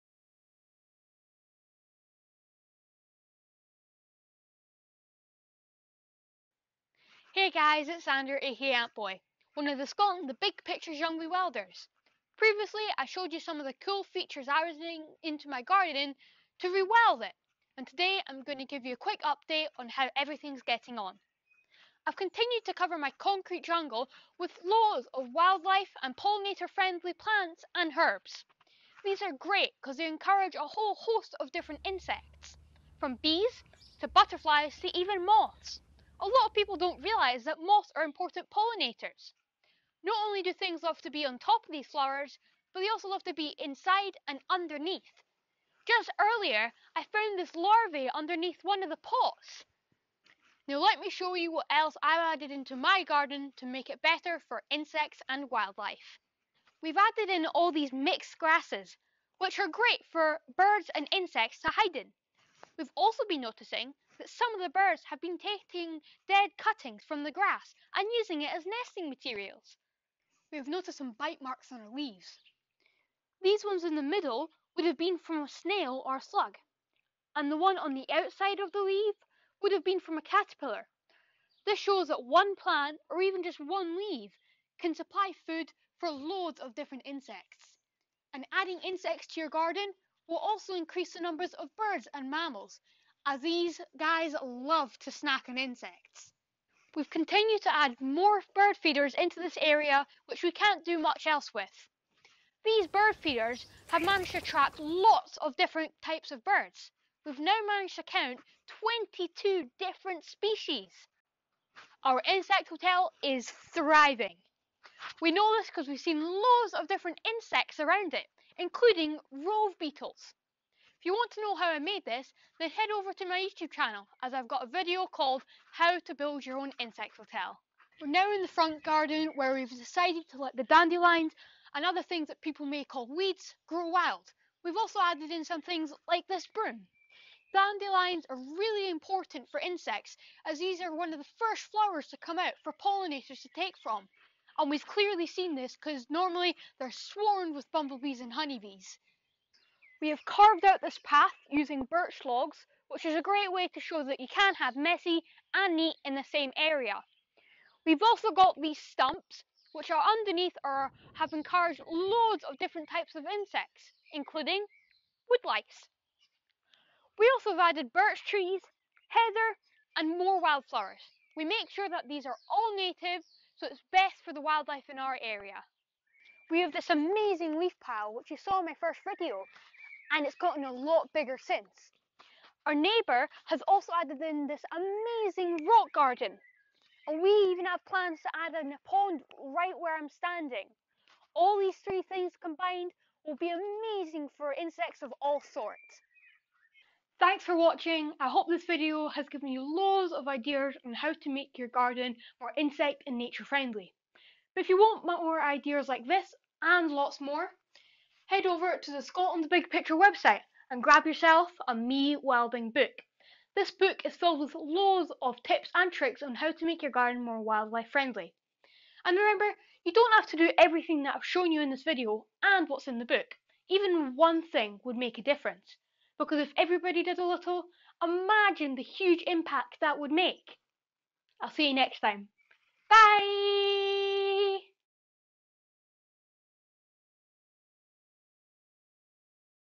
Xander is helping wildlife by rewilding his garden! How's he doing it?
"If everybody does a little, imagine what a huge impact that would make!"

The ever passionate STBP YoungRewilder, Xander Johnston, AKA Antboy, letting us know how he's rewilding his garden for the benefit of wildlife.

Find out more tips in our MeWilding book